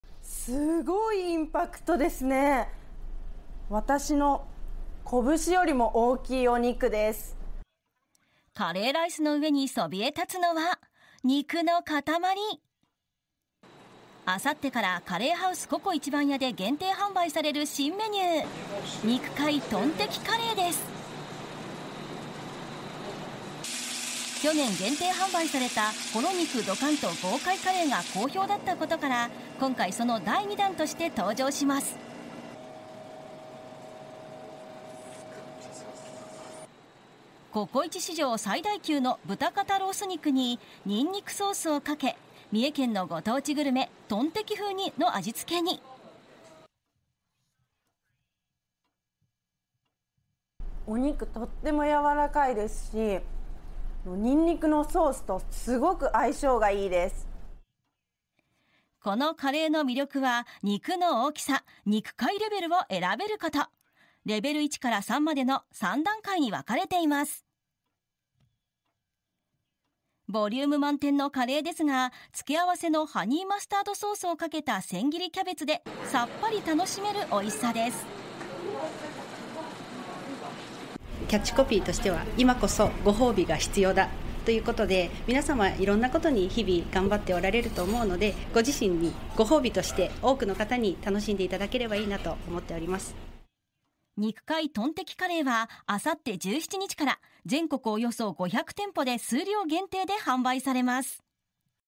ココイチに“豪快”新メニュー登場 肉の塊×トンテキでガツン！とくる食べごたえ (24/05/15 15:56)
ボリューム満点のカレーが食べたい人に朗報！愛知県一宮市が本社のカレーハウス「CoCo壱番屋」で、17日から期間限定メニューが販売されます。見た目も食べごたえもインパクト大です。

　カレーライスの上にそびえ立つのは、肉の塊。

　17日からカレーハウス「CoCo壱番屋」で限定販売される新メニュー、「肉塊トンテキカレー」です。

　去年、限定販売された「ホロ肉ドカンと豪快カレー」が好評だったことから今回、その第2弾として登場します。

　ココイチ史上最大級の豚肩ロース肉に、にんにくソースをかけ、三重県のご当地グルメ・トンテキ風の味付けに。

肉の大きさ＝「肉塊レベル」を選べる

　このカレーの魅力は、肉の大きさ＝「肉塊レベル」を選べること。レベル1から3までの3段階に分かれています。

　ボリューム満点のカレーですが、付け合わせのハニーマスタードソースをかけた千切りキャベツで、さっぱり楽しめるおいしさです。

　「キャッチコピーは『今こそ ごほうびが必要だ』。皆さん、いろんなことに日々頑張っているので、自分へのごほうびとして多くの方に楽しんでもらいたいと思っております」（壱番屋 販売促進部 吉田麻紀子さん）

　「肉塊トンテキカレー」は17日から、全国約500店舗で数量限定で販売されます。